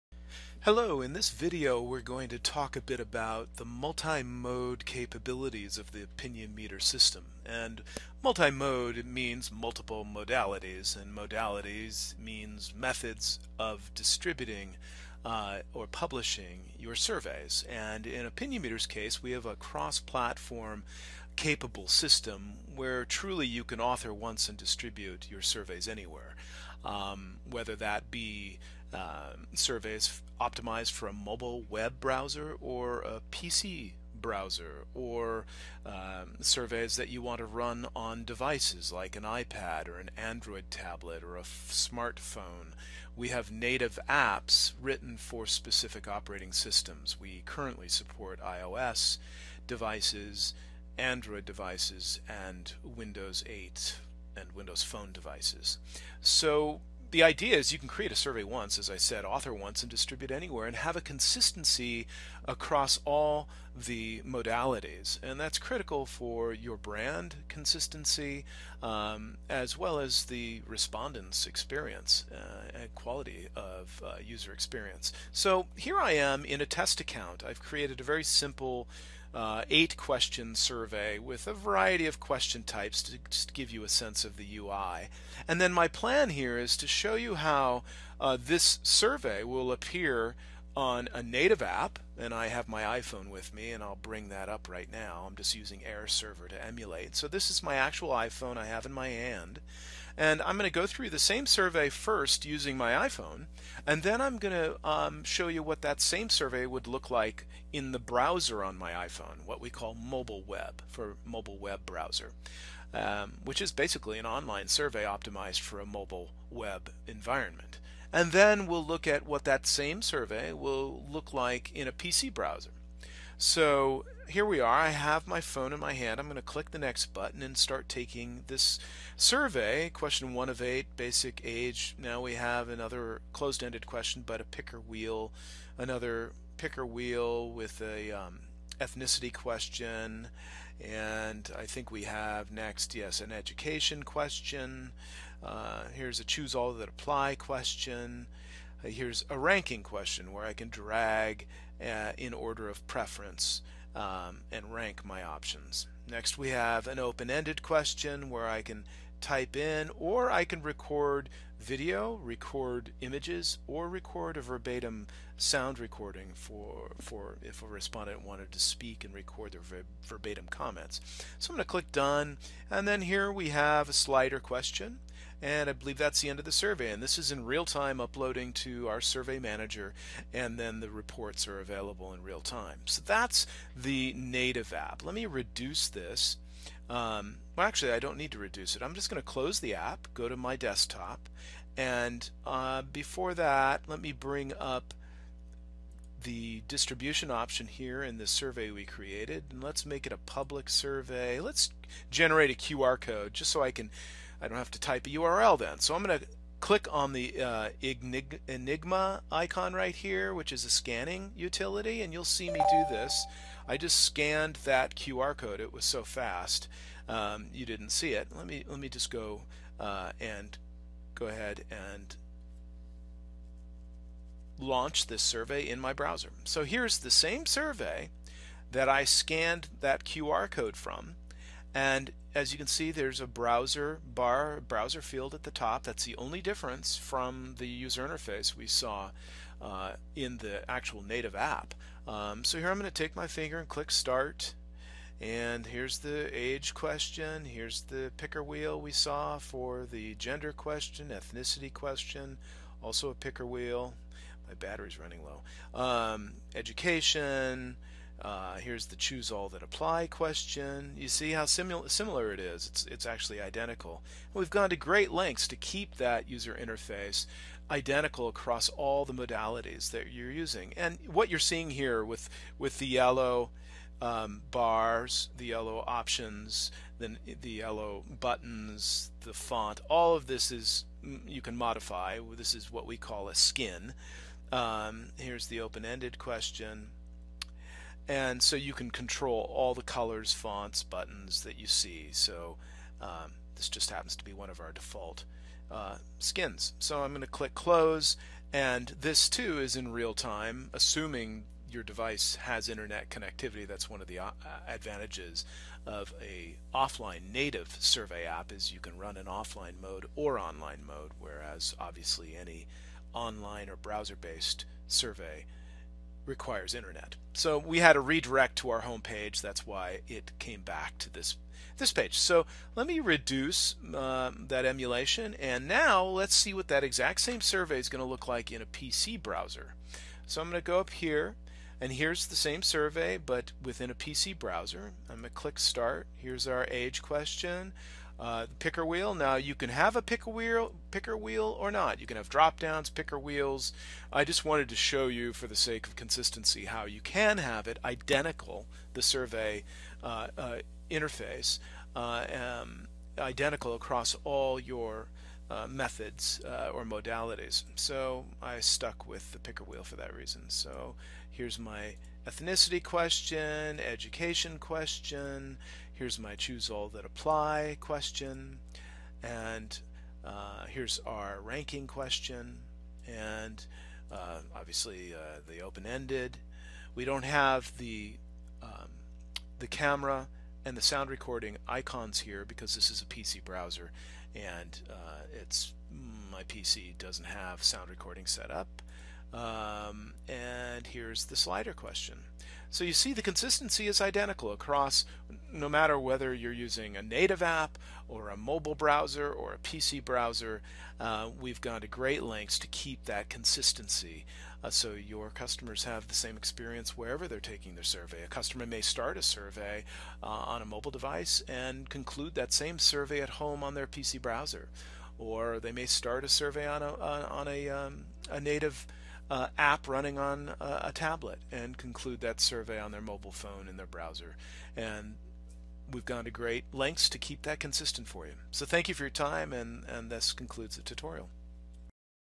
Cross-Platform Survey Platform: Author Once Distribute Anywhere
This video demonstrates Opinionmeter's cross-platform survey solution supporting multiple modalities from offline survey apps to mobile-web to PC browser interfaces. Now you can maintain the same branding consistency across multiple platforms and modalities.